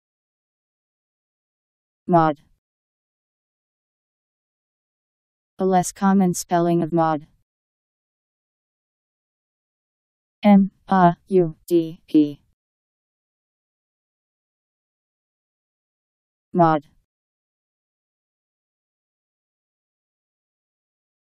Maude Meaning
Video shows what Maude means. , a less common spelling of Maud..  Maude Meaning. How to pronounce, definition audio dictionary. How to say Maude. Powered by MaryTTS, Wiktionary